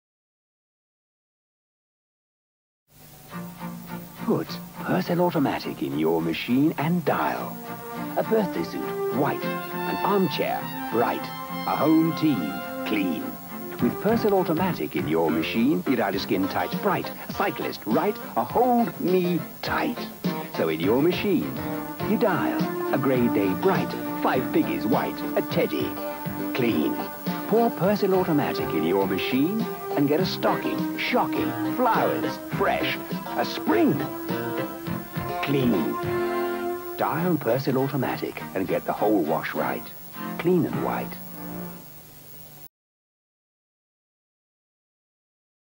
1984-1985: Persil' New System Automatic [Dial] - Persil Montage
Title: Dial
Brand: Persil
Advertiser/Client: Lever
Agency: -
Media: TV Advert
Featuring: -
Guest Appearance: -
Voice Over: -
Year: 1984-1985
Location: -
Country: UK
Washing Machine: New Generation Hotpoint
Model: -
Slogan: Whole Wash Right.

(Repost: Originally uploaded on The Laundry Lab, May 2018)